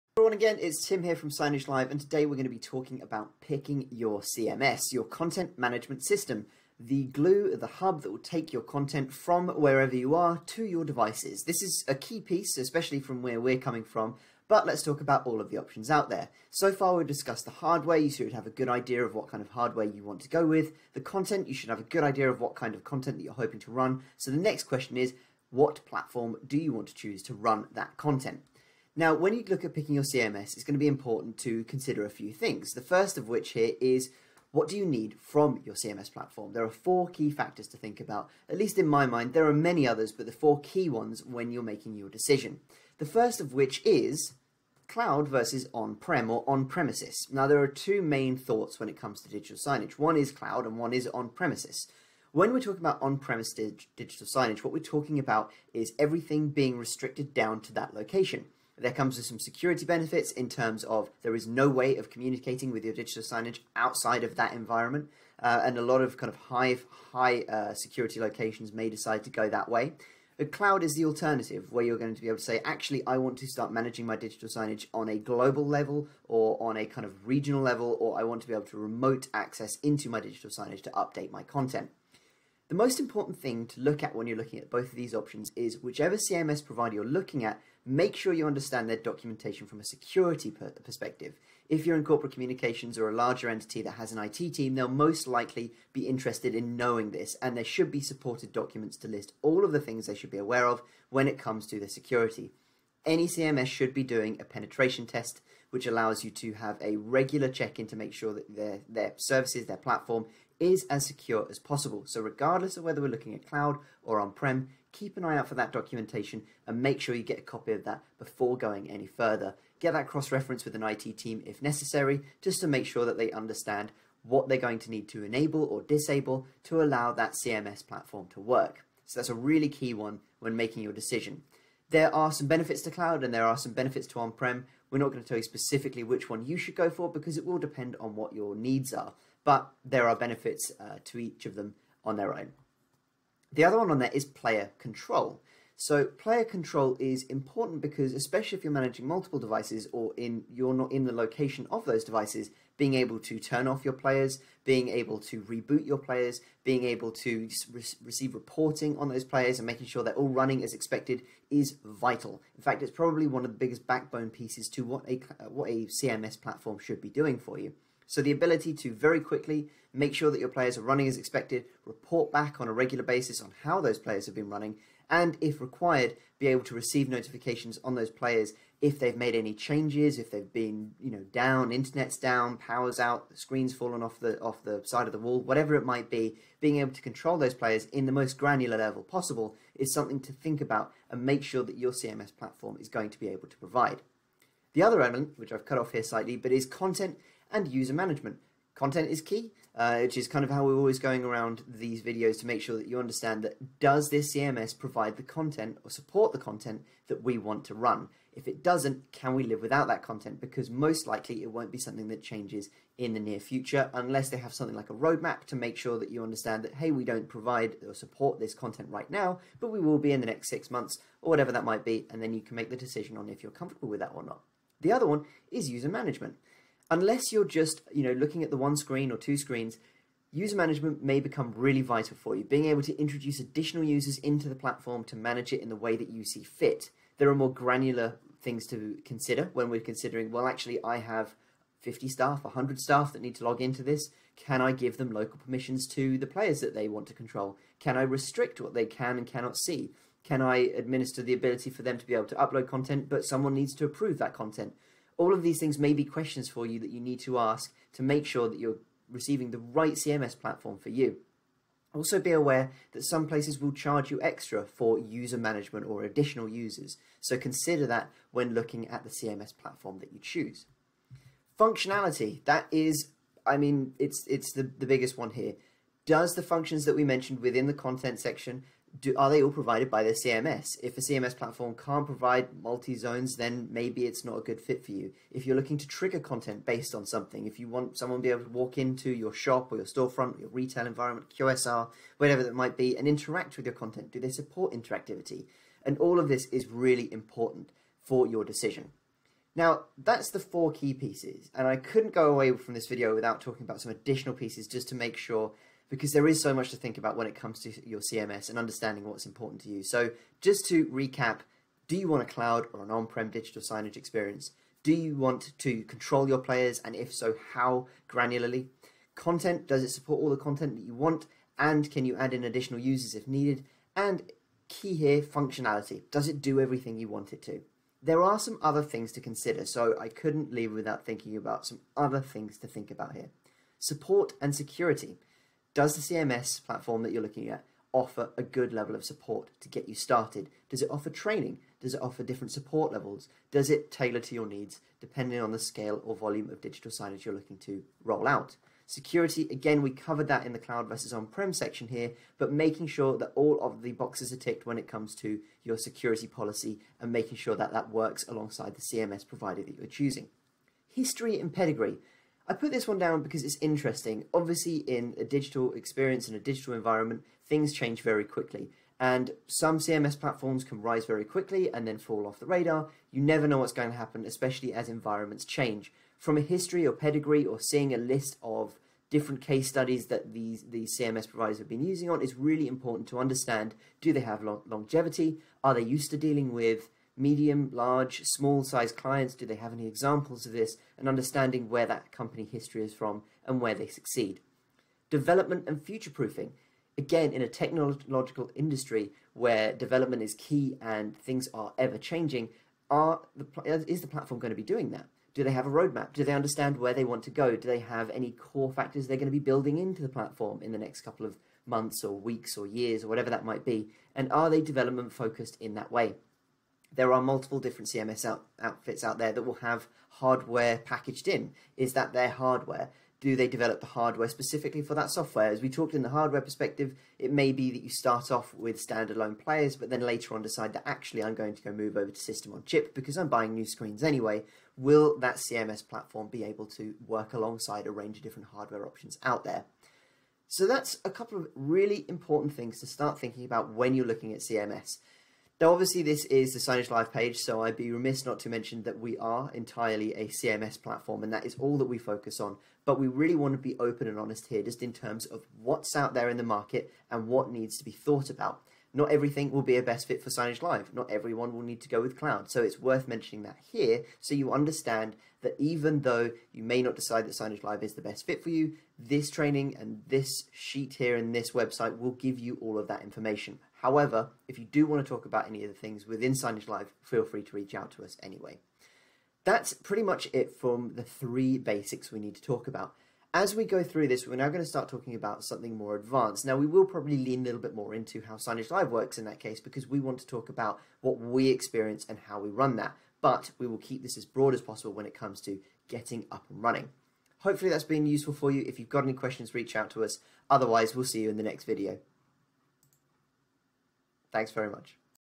What CMS do I need for digital signage?
When picking the correct digital signage CMS, you may be worried about selecting the wrong one. Take a look at this brief video to understand the functionality available in most CMS's and what you need to be aware of when picking the best one for you.